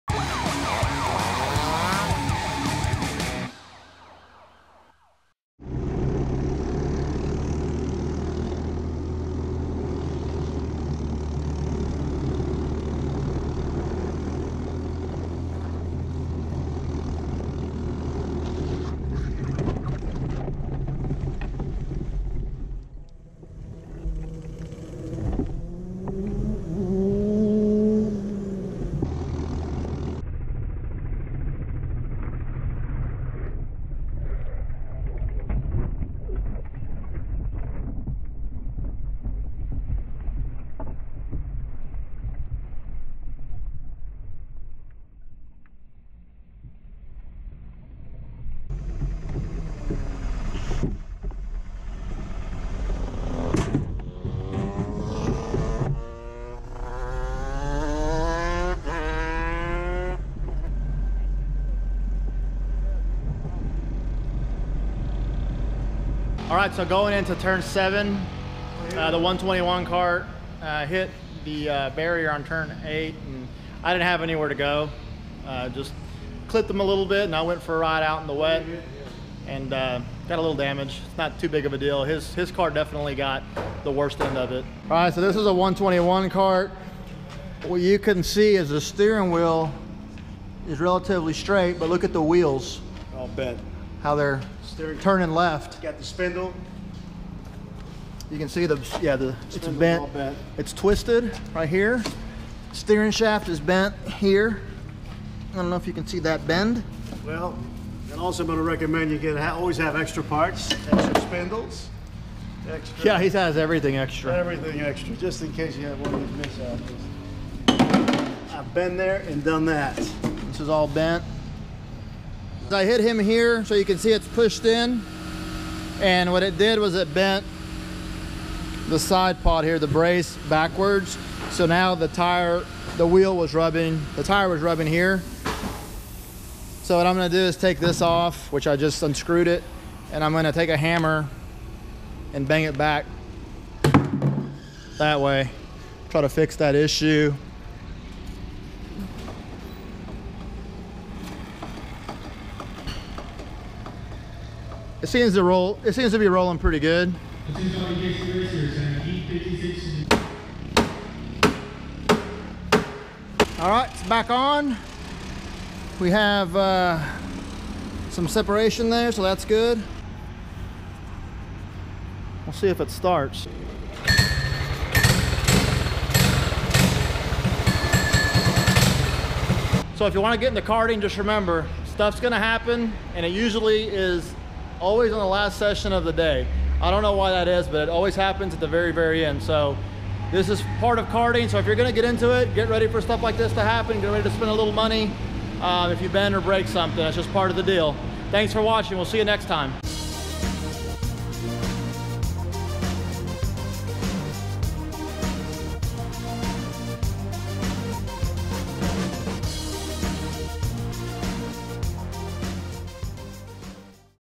Go Kart Crash in Unforgiving Turn (Damage included)
Me and the #121 kart crashed together during our final practice at Dallas Karting Complex before a race this weekend.  It always happens during your last session just before you're about to bring it into the pits for good.  Damage portion included.